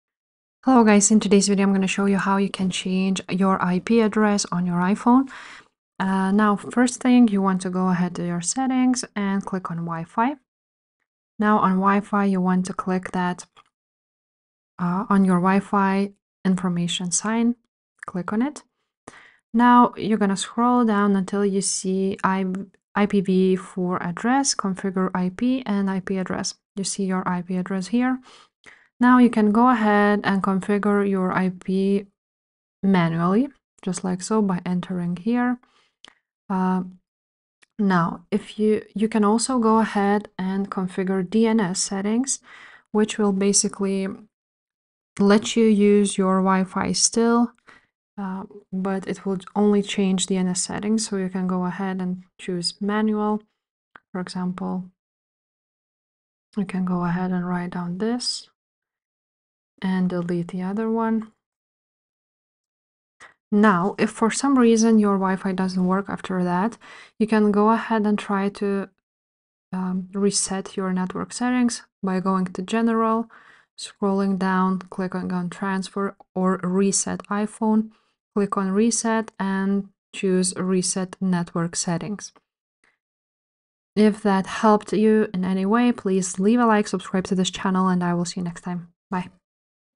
How To Change IP Address On iPhone
In this video I show you How To Change IP Address On iPhone!